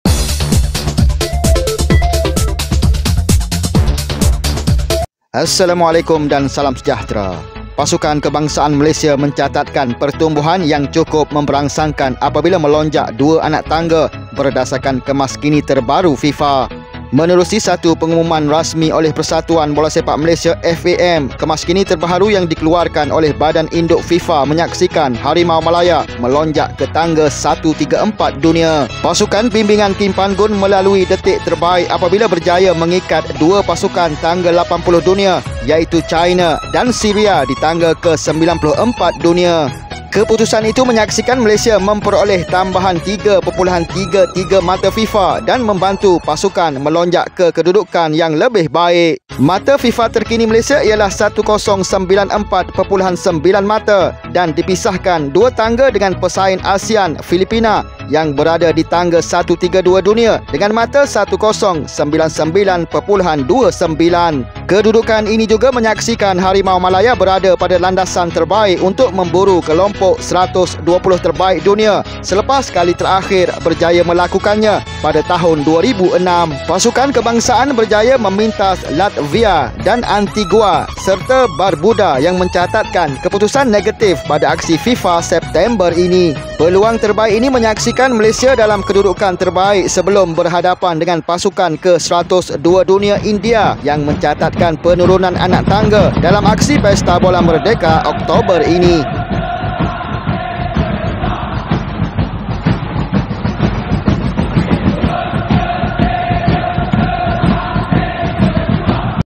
TERKINI "RANKING FIFA MALAYSIA" Kemaskini September 2023
TERKINI RANKING FIFA MALAYSIA NAIK LAGI

Pasukan kebangsaan Malaysia mencatatkan pertumbuhan yang cukup memberangsangkan apabila melonjak dua anak tangga berdasarkan kemas kini terbaru FIFA.

Menerusi satu pengumuman rasmi oleh Persatuan Bola Sepak Malaysia (FAM), kemas kini terbaru yang dikeluarkan oleh badan induk FIFA menyaksikan Harimau Malaya melonjak ke tangga 134 dunia.

Pasukan bimbingan Kim Pan-gon melalui detik terbaik apabila berjaya mengikat dua pasukan tangga 80 dunia iaitu China dan Syria di tangga ke-94 dunia.

Keputusan itu menyaksikan Malaysia memperoleh tambahan +3.33 mata FIFA dan membantu pasukan melonjak ke kedudukan yang lebih baik.

Mata FIFA terkini Malaysia ialah 1094.9 mata dan dipisahkan dua tangga dengan pesaing ASEAN, Filipina yang berada di tangga 132 dunia dengan 1099.29 mata.

Kedudukan ini juga menyaksikan Harimau Malaya berada pada landasan yang terbaik untuk memburu kelompok 120 terbaik dunia selepas kali terakhir berjaya melakukannya pada tahun 2006.

Pasukan kebangsaan berjaya memintas Latvia dan Antigua dan Barbuda yang mencatatkan keputusan negatif pada aksi FIFA September ini.

Peluang terbaik ini menyaksikan Malaysia dalam kedudukan terbaik sebelum berhadapan dengan pasukan ke-102, India yang mencatat penurunan anak tangga dalam aksi Pestabola Merdeka, Oktober ini.
KREDIT
ARTIKEL RUJUKAN : MAKAN BOLA
FOTO : FA MALAYSIA